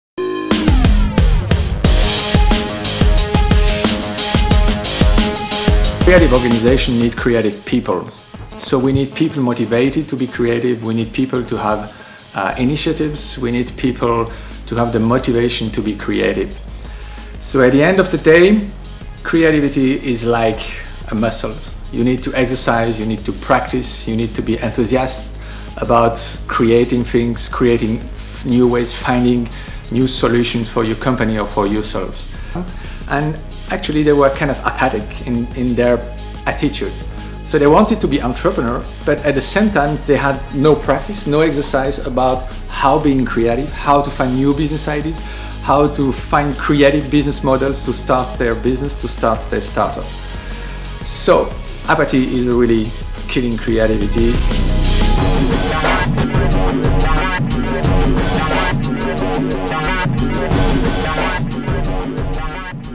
WKC Expert -Prof Antione Perruchoud on Who Killed Creativity?
Prof Antion Perruchoud: Manager of the Entrepreneur School of Business Studies Western University of Switzerland explains why 'the Apathy Clan ' is a prime suspect in the  Who Killed Creativity- Crime Scene Investigation.